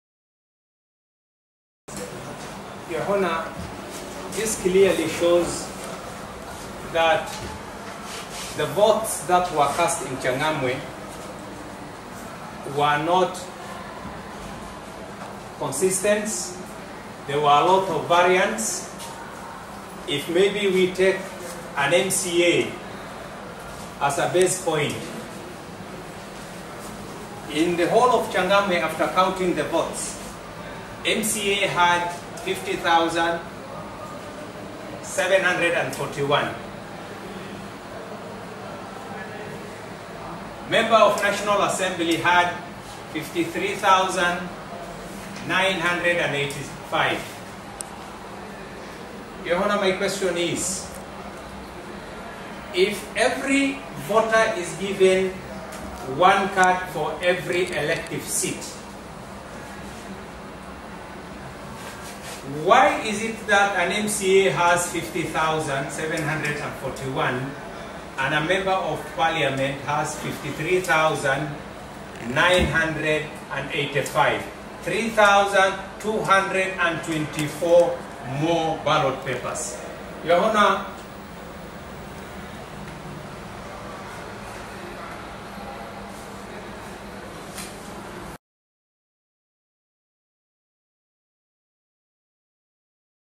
Votes in Changamwe had a lot of variance- Jubilee Aspirant tells court
Changamwe Constituency Jubilee Aspirant Abdi Daib told the Mombasa Court that the votes casted in the area on the 8th August 2017  had a lot of variance, a situation which raised eyebrows on its legality. He was testifying in a petition he filed before a Mombasa Law Court challenging the Election of Omar Mwinyi as the area MP.